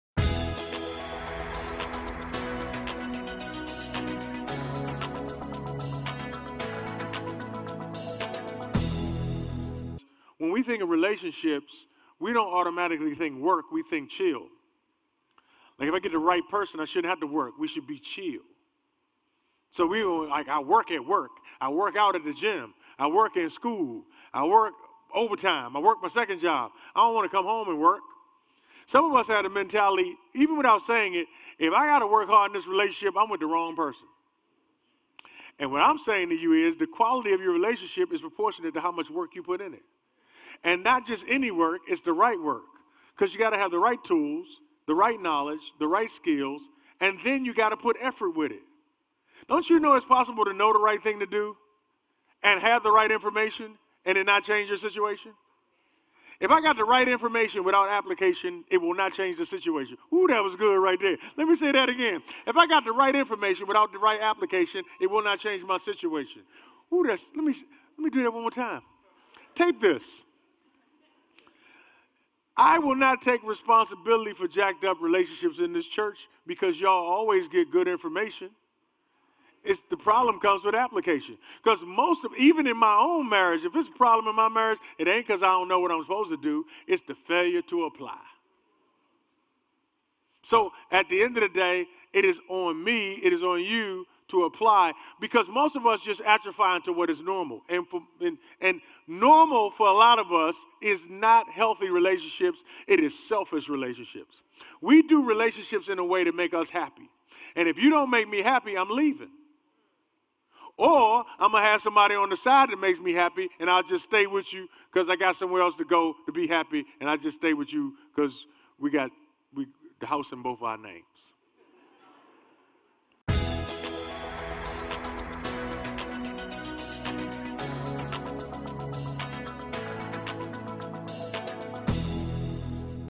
Sermon Highlight from "Proverbial Pt.2"
Excerpt from "Proverbial Pt.2" with Pastor Keith Battle. To view this message in it's entirety please